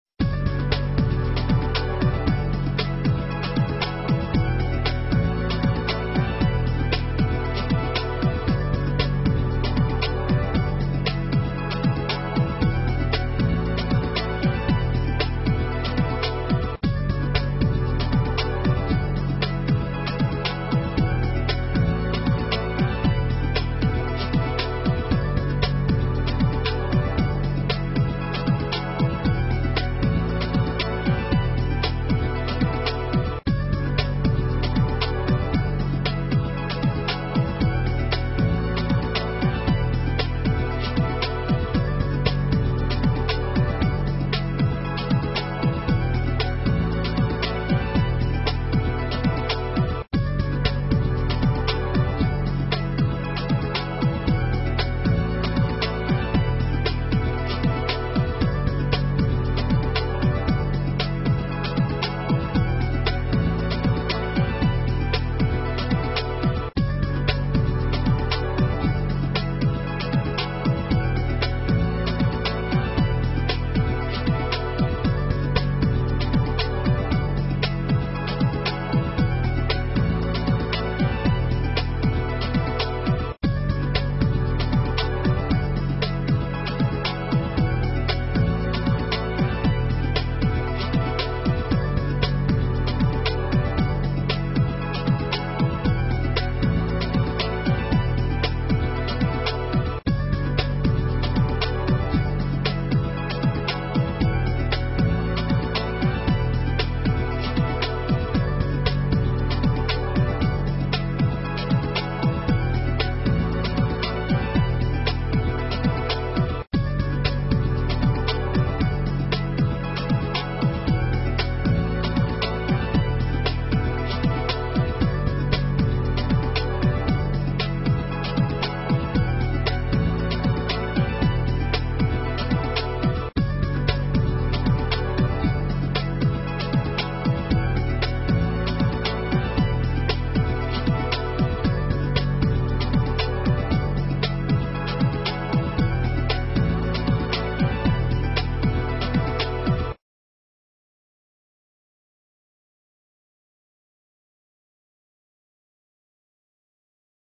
Dental Plans in Arizona
- There are two types of dental plans available in Arizona and the people living there should know the differences between them before they buy. Watch this short video right away and then click the link above to find out more.